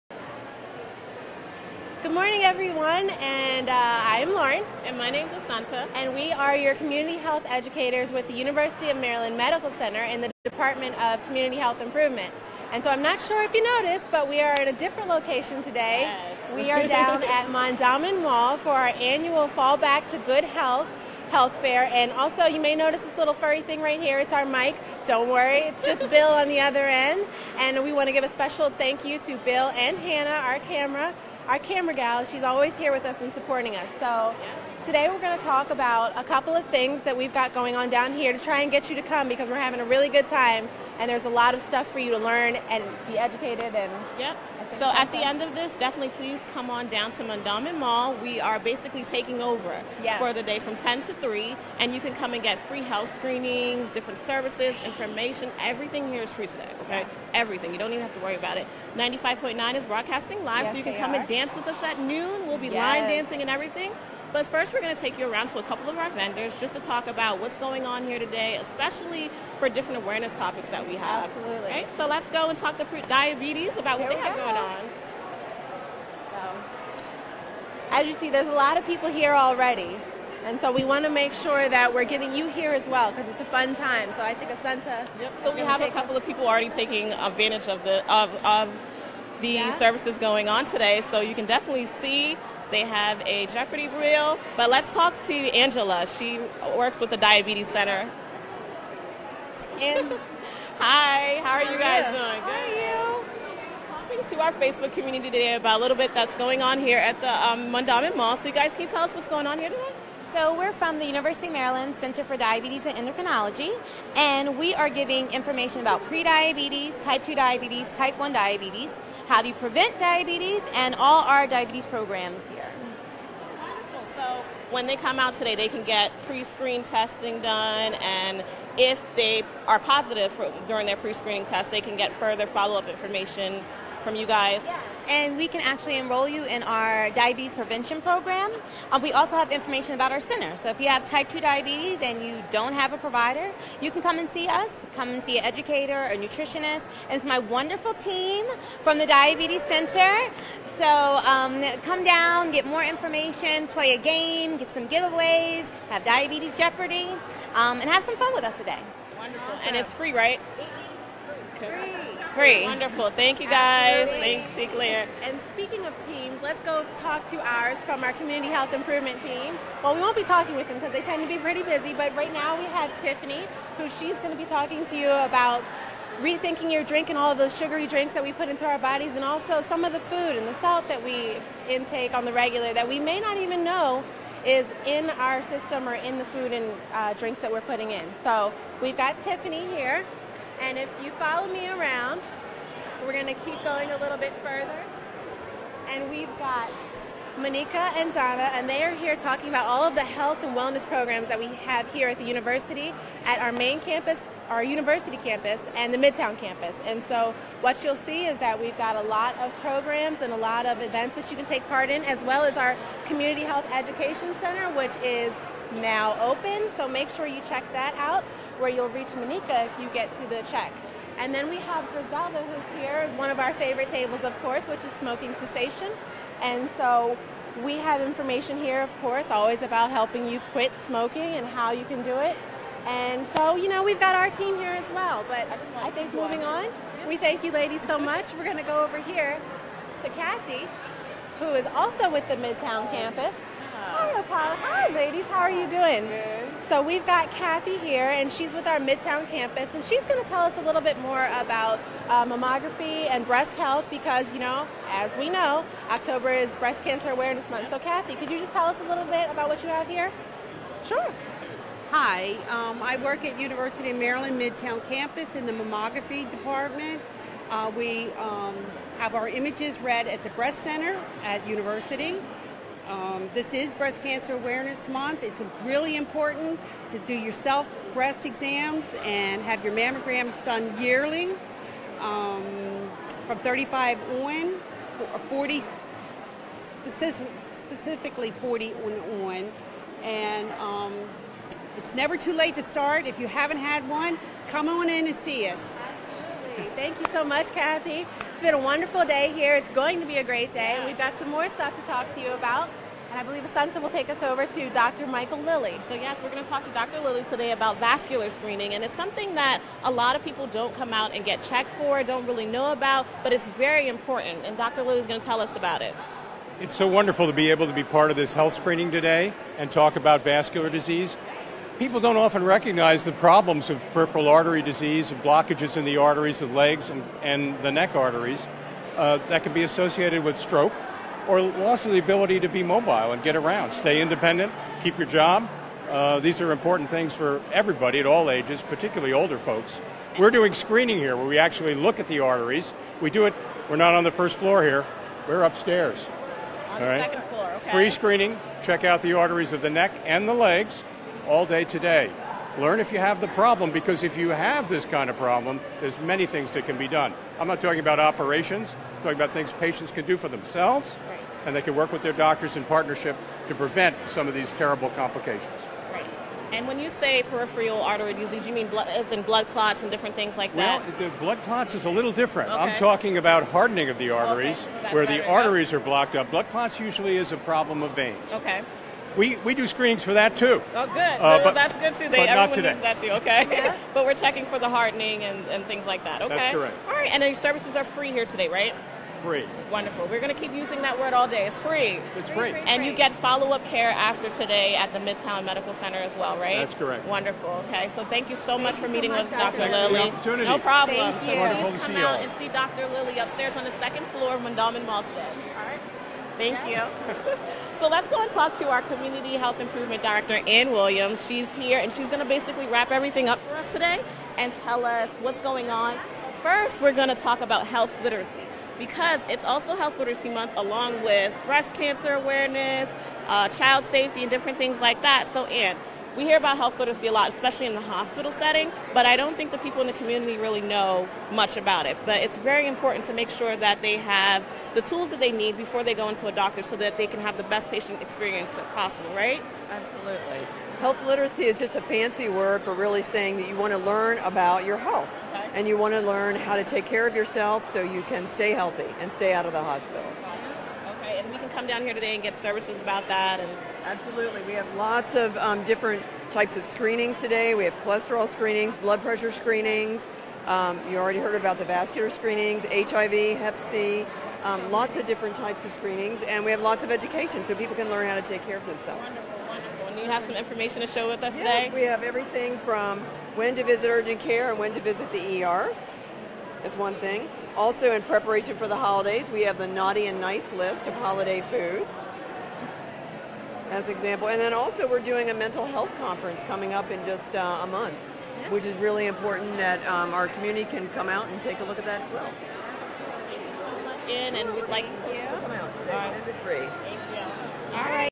Fall Back to Good Health Fair | Community Health Facebook Live
We were live at the Fall Back to Good Health Fair at Mondawmin Mall! There's truly something for everyone - with free screenings, fitness demonstrations, community and work-force development resources and health & wellness information.